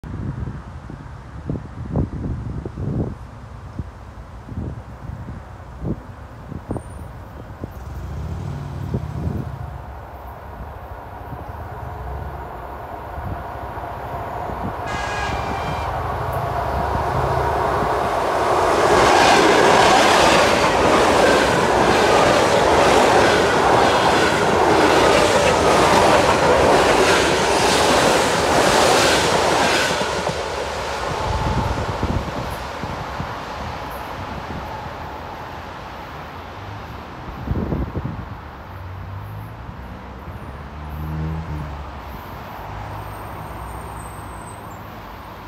ЭД4М-0290 На перегоне Лосиноостровская-Ростокино
ОПИСАНИЕ

Эксплуатация:
С завода поступил в депо Москва-II Московской ж. д.

До 11-вагонной составности был дополнен вагоном 016705, затем 019605; 026611; 017707

Вагоны и сетевые коды:
01 — 13458658
02 — 13458666
03 — 13458674
04 — 13458682
05 — 13458690
06 — 13458708
07 — 13458716
08 — 13458724
09 — 13458732
10 — 13458740

Ремонт:
08.2016 — ТР-3 депо Данилов
05.2019 — КР-1 Московский ЛРЗ (без вагона 016705)